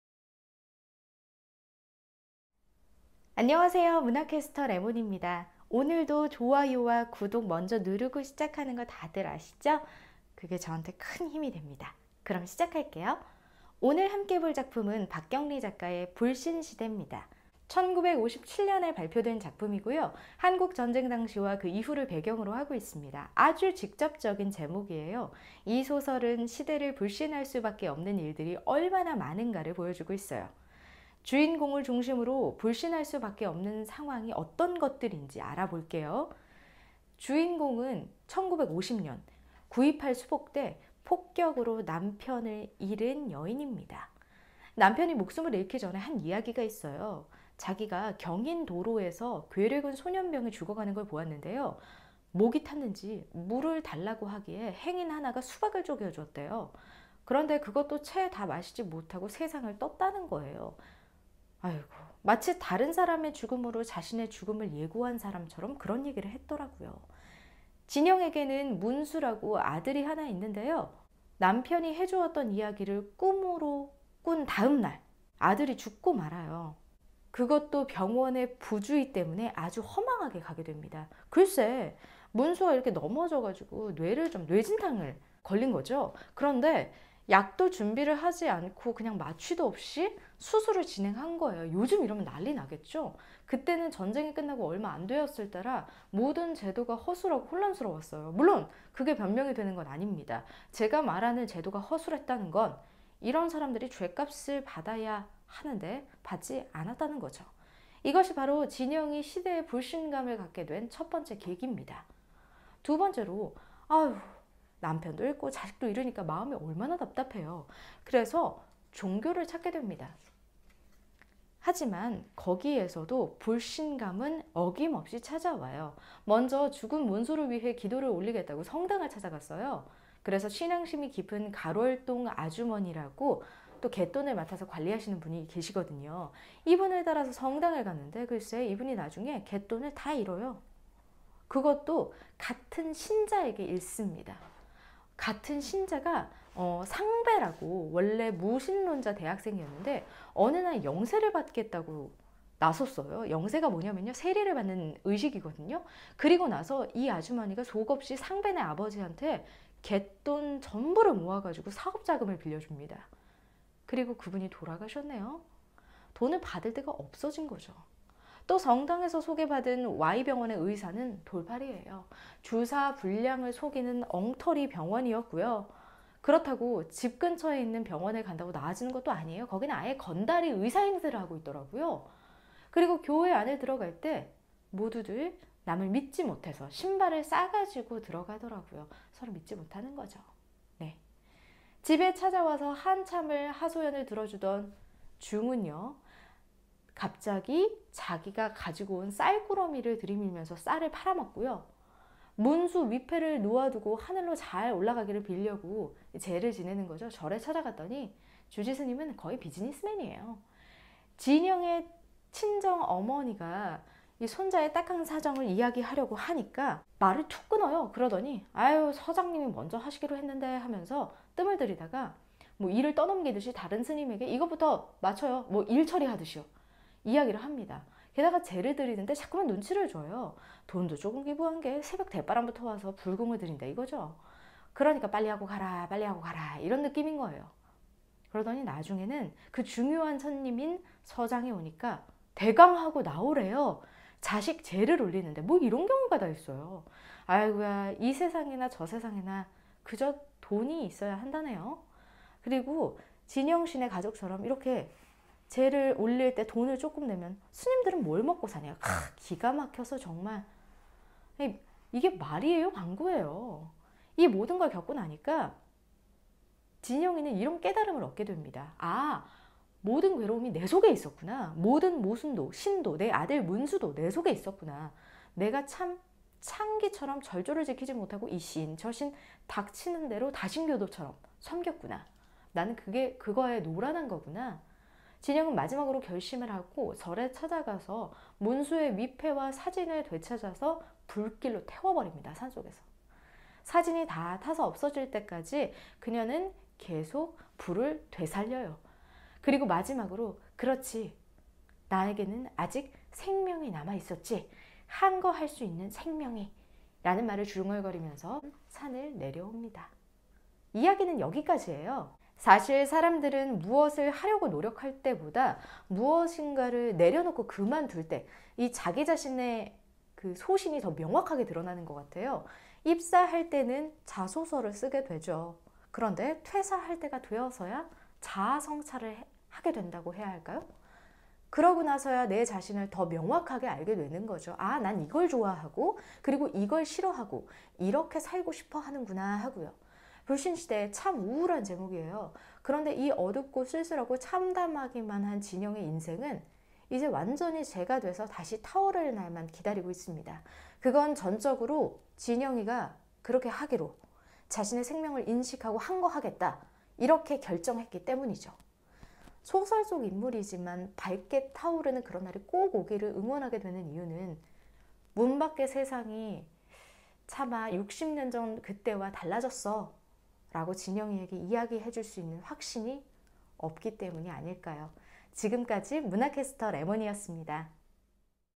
[10분의 문학] 제25화 불신시대 박경리 전문해설
누구도 믿을 수 없는
박경리작가 의 슬프고도 처절한 진실 탐구!
문학 신청하기, 질문하기는 댓글로!:)